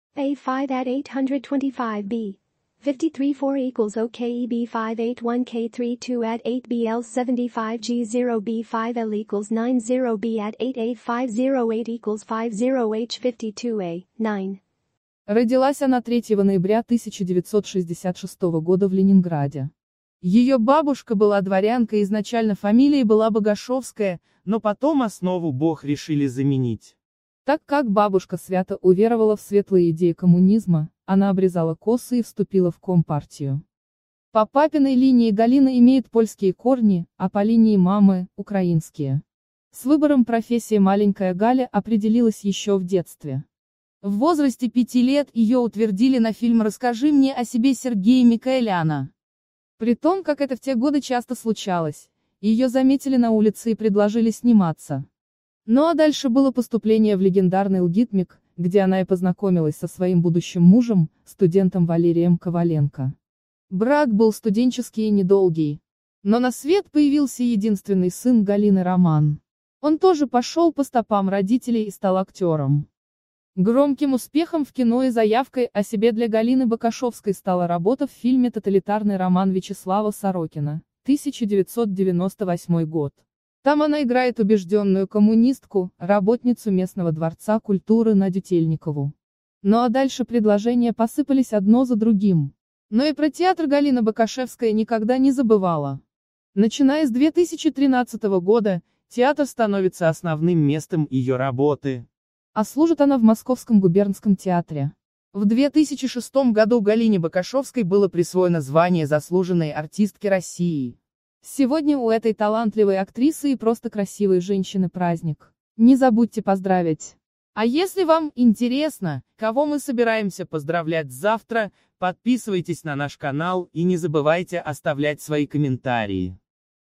Тоталитарный роман Галины Бокашевской.
Всем привет!  Сегодня мы хотели бы поговорить о замечательной актрисе    Галине Бокашевской.